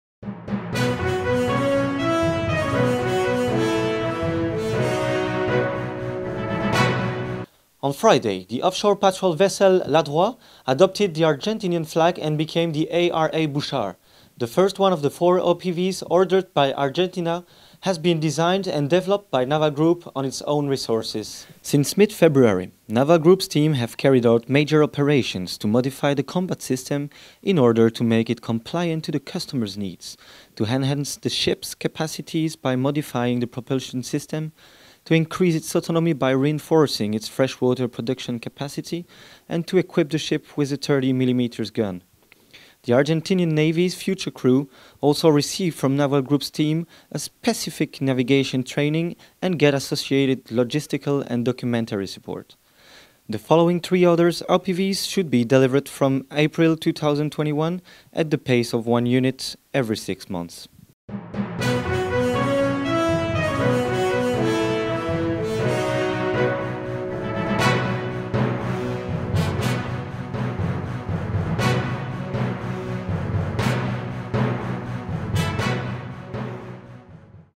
Offshore Patrol Vessel OPV 87 L'Adroit becomes A.R.A. Bouchard of Argentina Navy Naval Group
The Offshore Patrol Vessel (OPV 87) L’Adroit adopts the Argentinian flag and becomes the A.R.A. BOUCHARD. The first one of the four OPVs ordered by Argentina has been designed and developed by Naval Group on its own resources.